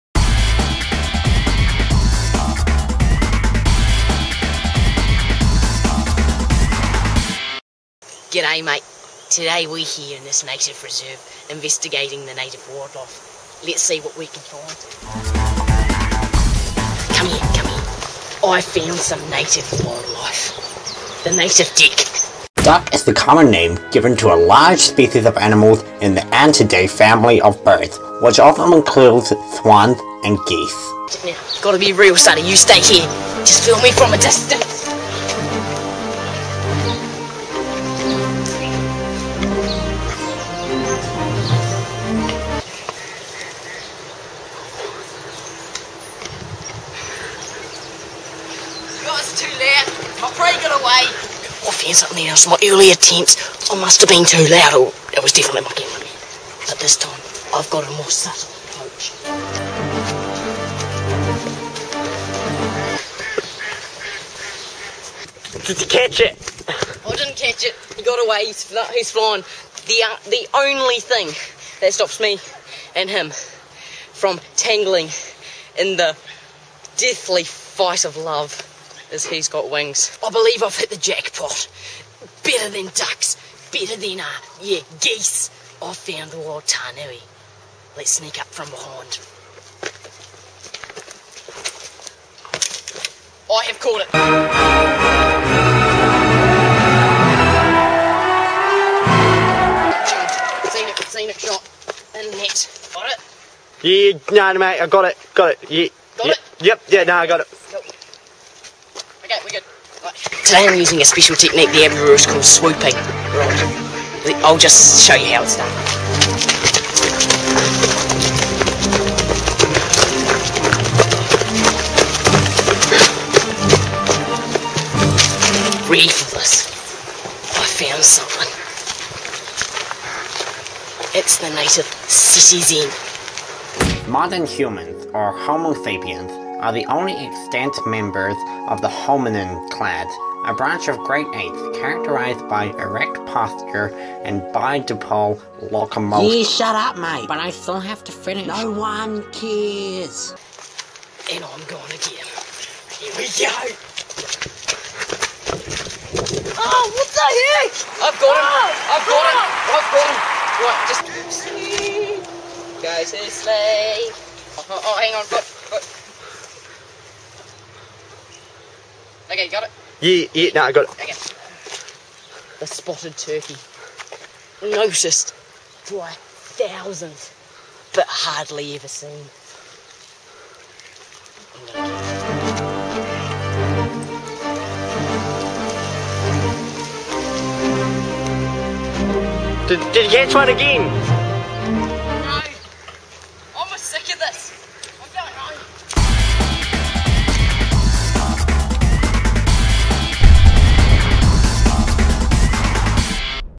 Sam Urwin- Duck Wrangler
Sam Urwin is out today trying to hustle with the native wild life at a local lake reserve. This is a parody of the late Steve Irwin, the crocodile hunter. We had to change it because crocodiles aren't easy to find in New Zealand..